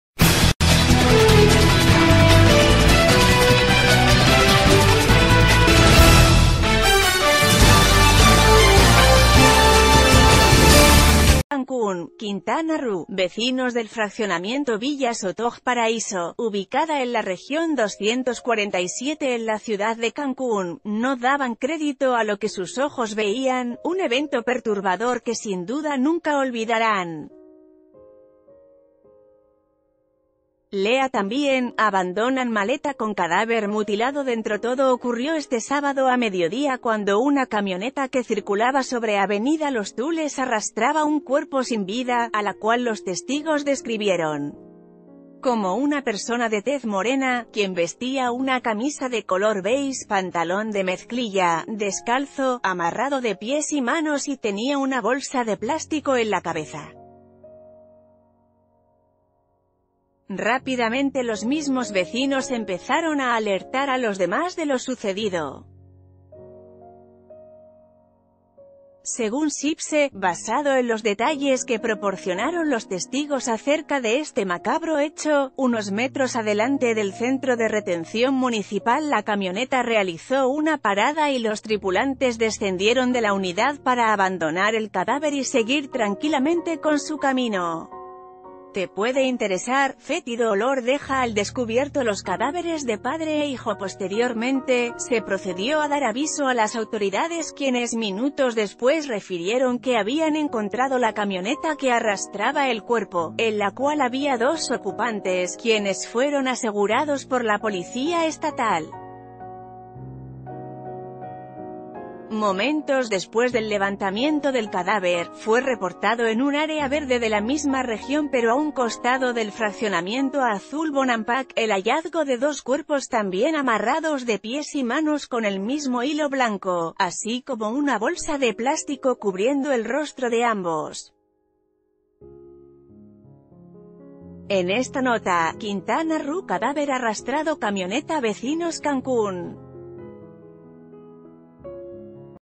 Camioneta arrastra cadáver, testigos se quedan "helados"
Cancún, Quintana Roo.- Vecinos del fraccionamiento Villas Otoch Paraíso, ubicada en la Región 247 en la ciudad de Cancún, no daban crédito a lo que sus ojos veían, un evento perturbador que sin duda nunca olvidarán. Lea tambíén: Abandonan maleta con cadáver mutilado dentroTodo ocurrió este sábado a ...